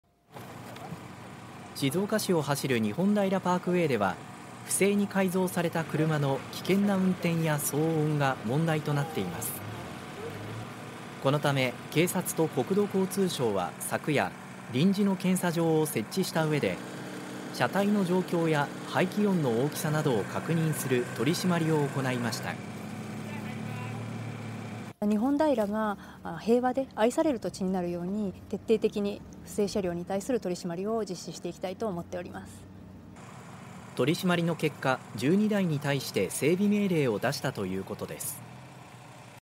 不正改造は許さない！危険な運転や騒音で苦情相次ぐ市道で警察と国交省が”車検”を実施 静岡
不正に改造された車による危険な運転や騒音に対して多くの苦情が寄せられている静岡市の市道では、6月21日夜、警察などによる取り締まりが行われました。

静岡市を走る日本平パークウェイでは、不正に改造された車の危険な運転や騒音が問題となっています。

このため警察と国土交通省は6月21日夜、臨時の検査場を設置した上で車体の状況や排気音の大きさなどを確認する取り締まりを行いました。

清水警察署・増田明香 地域交通官：
日本平が平和で愛される土地になるように、徹底的に不正車両に対する取り締まりを実施していきたいと思っております

取り締まりの結果、計12台に対して整備命令を出したということです。